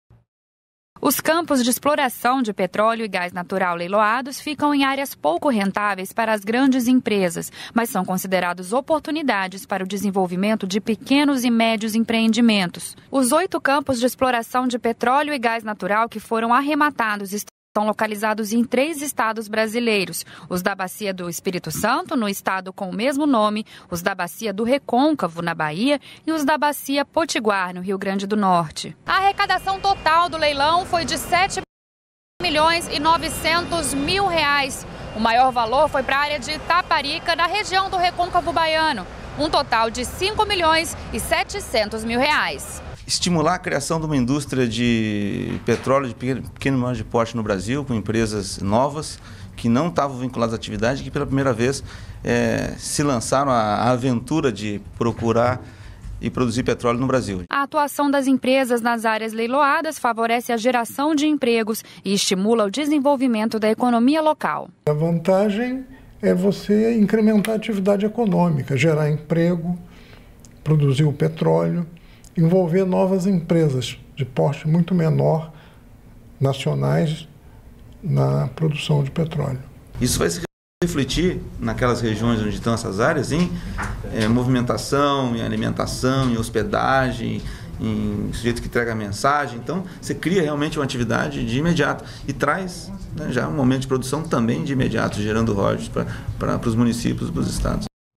Governo faz leilão de campos de petróleo
REPÓRTER NBR - 11.05.17: A Agência Nacional de Petróleo, Gás Natural e Biocombustíveis (ANP) realizou a 4ª Rodada de licitações de áreas com acumulações maduras. Os campos de de exploração de petróleo e gás natural leiloados ficam em áreas pouco rentáveis para grandes empresas, mas são consideradas oportunidades para pequenos e médios empreendimentos.